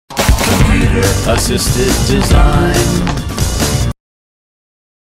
Computer Assisted Design Part 1? for Zorobutt
ALRIGHT GETTING MAP PARTS DONEE
Sorry for the lack of animation. I hope it's alright. Anyway, this is one of my OCs, Raimu. He's some kind of alien critter (he might be a timecat lol) and he's always coming up with crazy devices that fail horribly.
Poor guy.
AND HIS DESIGN IS HARD TO ANIMATE hHHNG